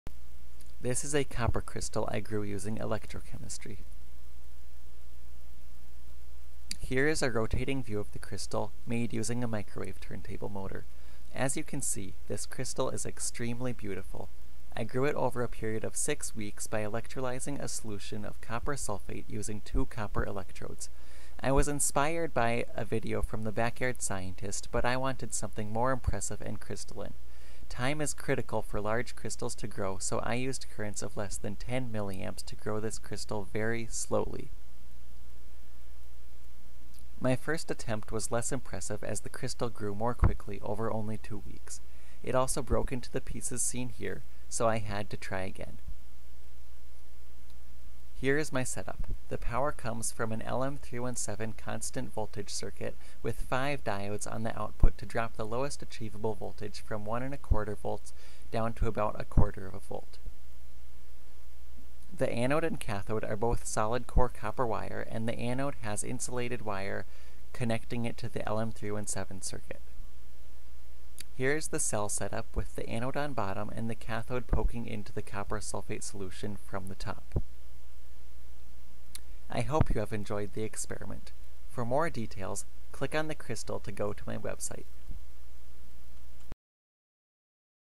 Grow Copper Metal Crystals with Electrochemistry!
This is a 1x1x1.5cm copper metal crystal I grew using electrolysis of copper sulfate with two copper electrodes.  The solution concentration was 40g/L, and the current was always kept at less than 10mA so that this entire crystal grew over six weeks.  The finished crystal was put in mineral oil to protect its pink shine.

If you have questions, don't hesitate to comment or use my website's contact page!  I WILL reply with as much help as possible!